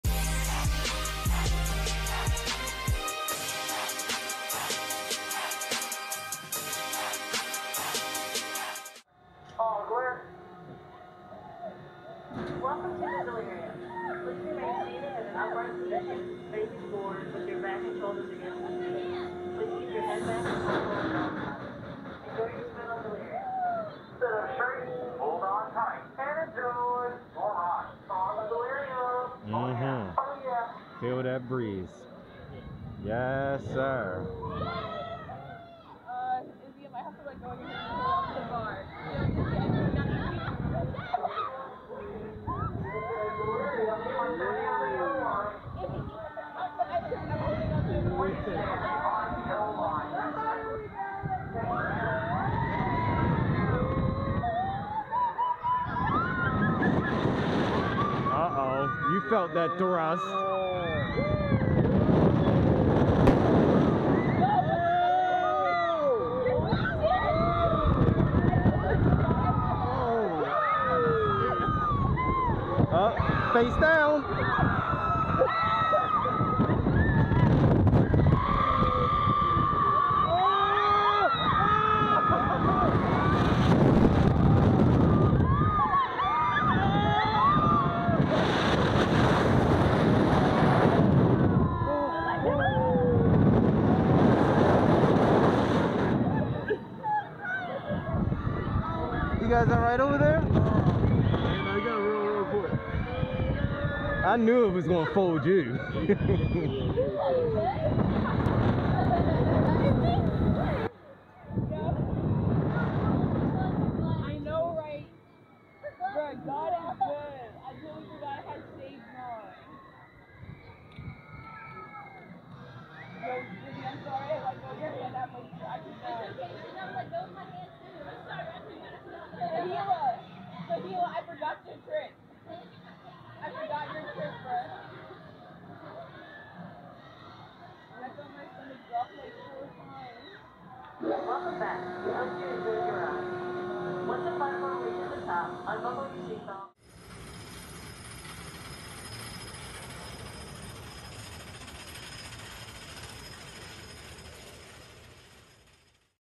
Delirium Kings Dominion on ride POV
FlatRideFriday has come to a conclusion with honestly one of my least favorite flat rides. Delirium at Kings Dominion has a complicated loading as well as dispatch system which makes the ride itself underwhelming because you waited so long per cycle. I wanted this spot for FINNEGANS FLYER but we had a SIX FLAGS DAY at Busch Gardens Williamsburg video coming soon.